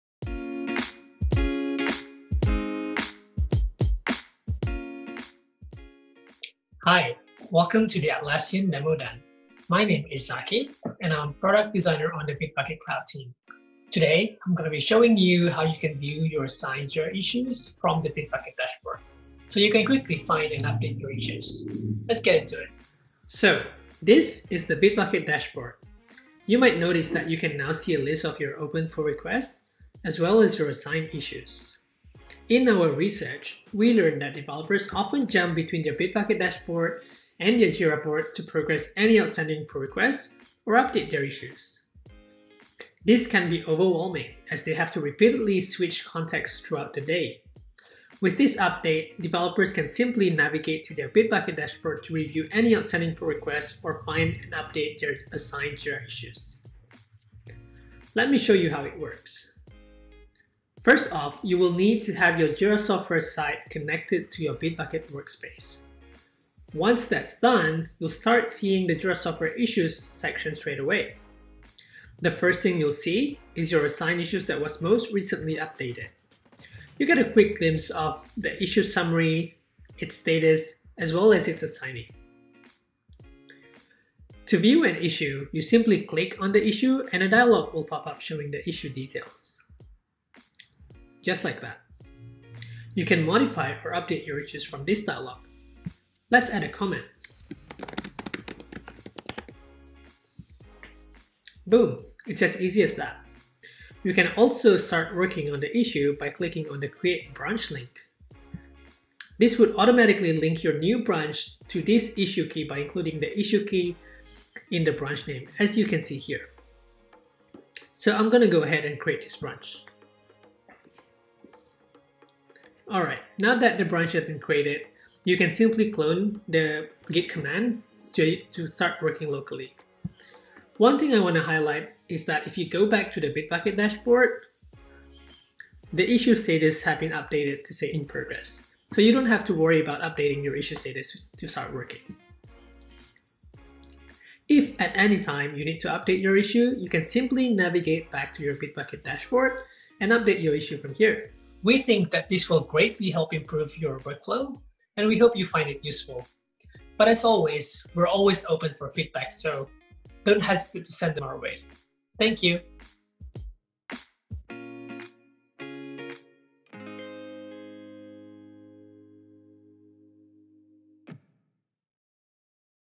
Jira Issues on your Bitbucket Dashboard - Demo Den
Bitbucket Cloud team Product Designer demos how you can view your assigned Jira issues on your Bitbucket dashboard. Check out this feature to learn how to help your team quickly find, pick up and update your issues! Connect your tools with Atlassian Open DevOps

The Demo Den is a regular feature demo by Atlassian Product Managers, Engineers, and Product Designers.